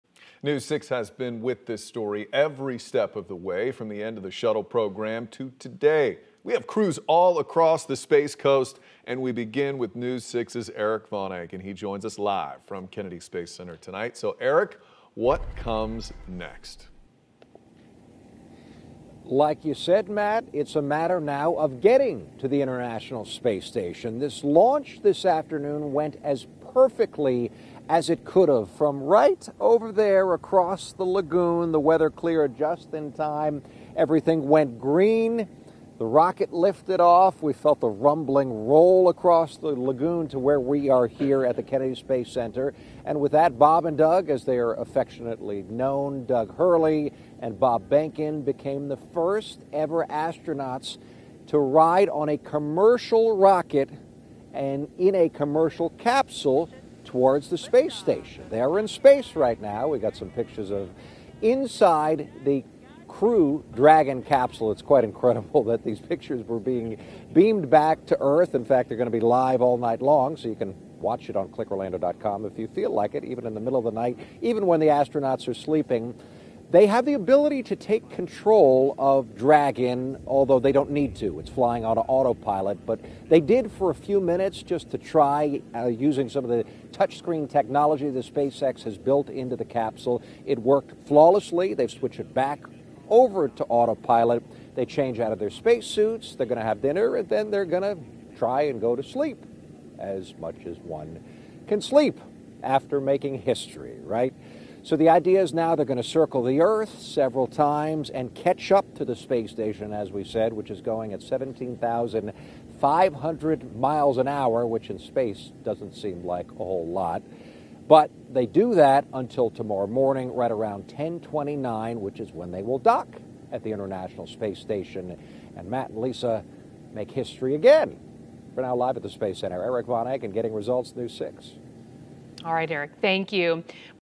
Astronauts two & a half hours into mission
Erik VonAncken reporting at 6pm from Kennedy Space Center